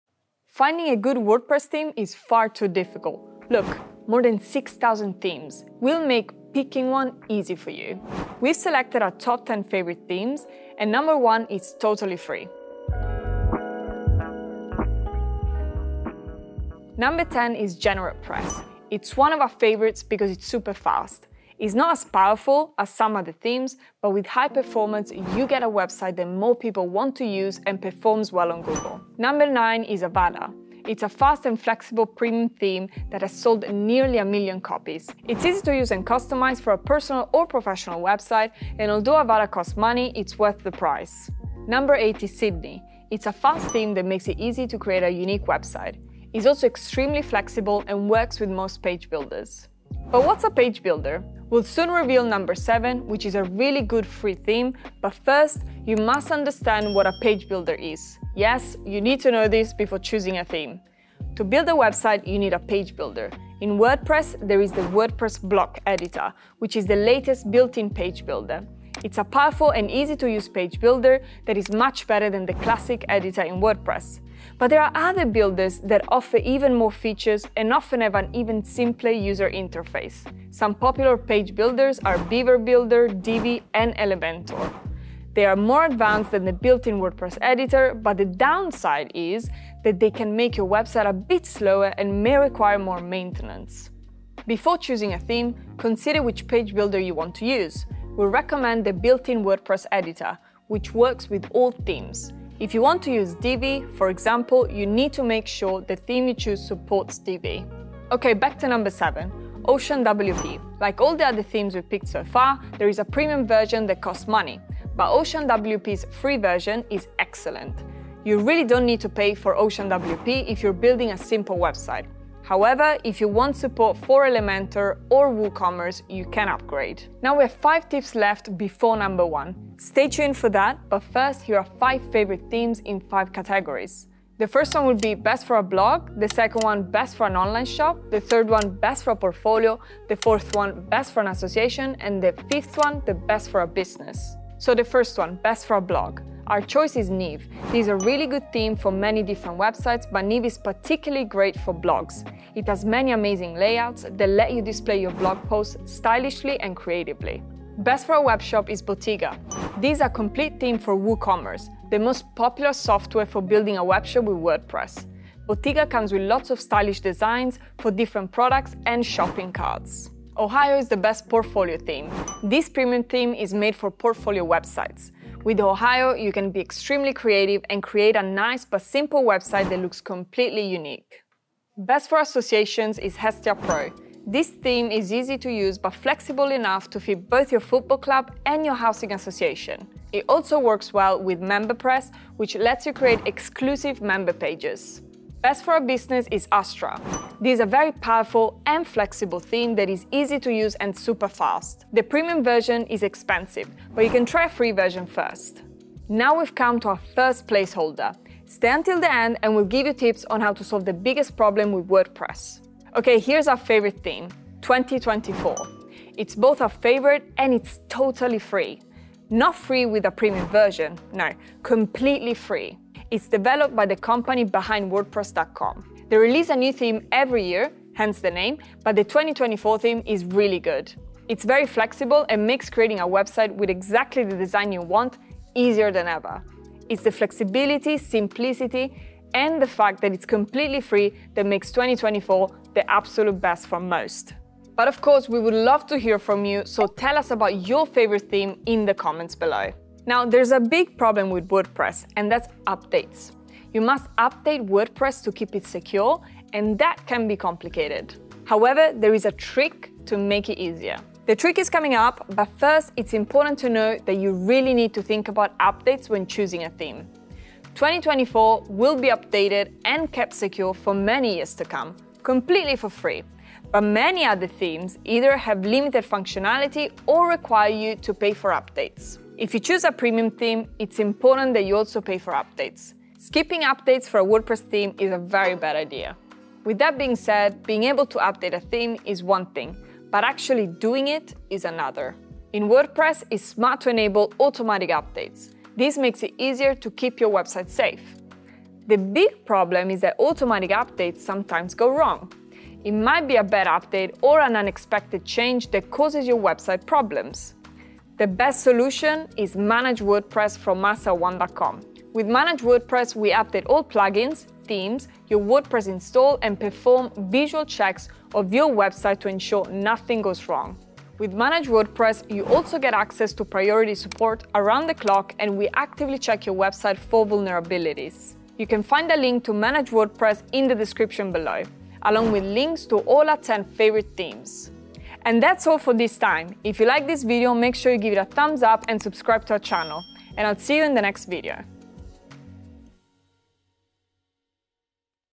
Top 10 best WordPress themes in 2025 (Our #1 pick is FREE!)
Looking for the best WordPress themes to take your website to the next level? In this video, we’re breaking down the top 10 WordPress themes for 2025, whether you’re building a blog, portfolio, business site, or online store!